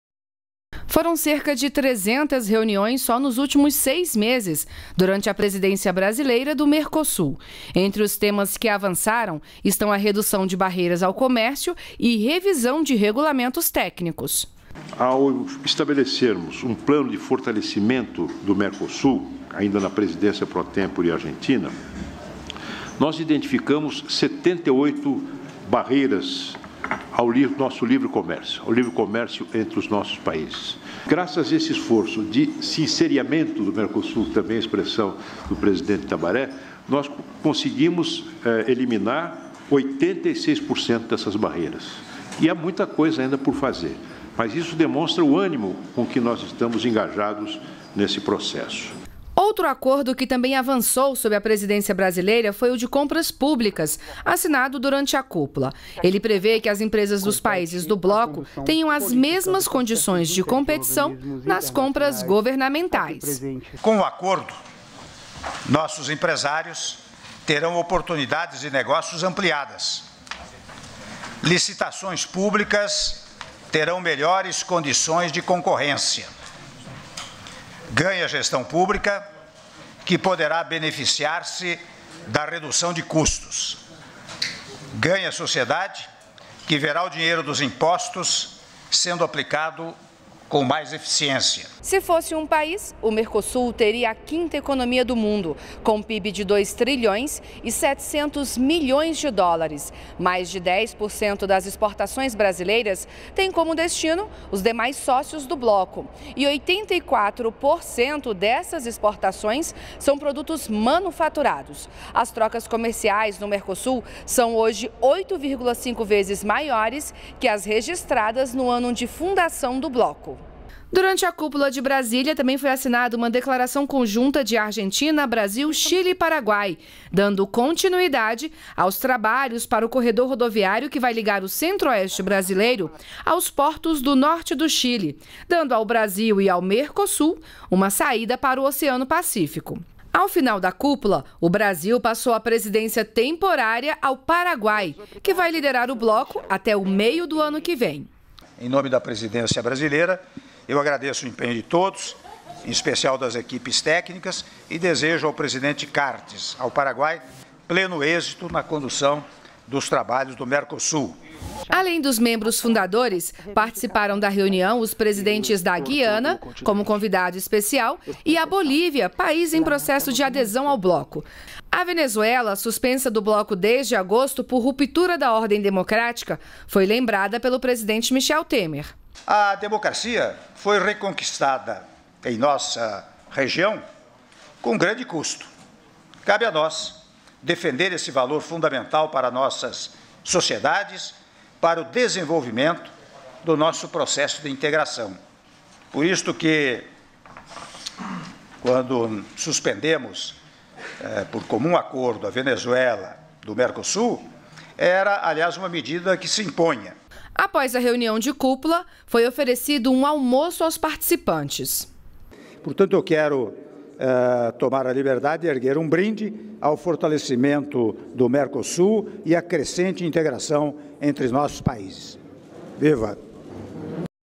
Acordos de redução de barreiras avançam com o Brasil na presidência do Mercosul
REPÓRTER NBR - 22.12.17: Durante a presidência brasileira do Mercosul, foram feitas 6 reuniões. Entre os temas que avançaram no bloco, estão a redução das barreiras do comércio e revisão de regulamentos técnicos.